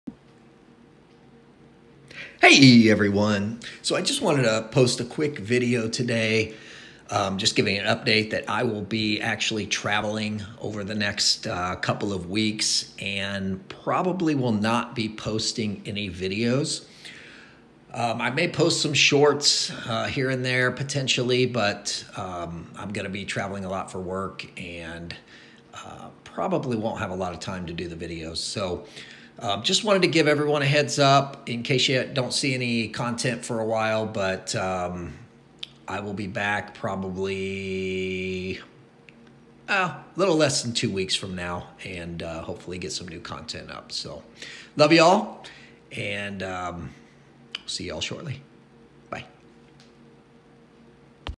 Gone For Two Weeks - No New Videos
Will be traveling for work and won’t be posting any new videos during the next two weeks

God Bless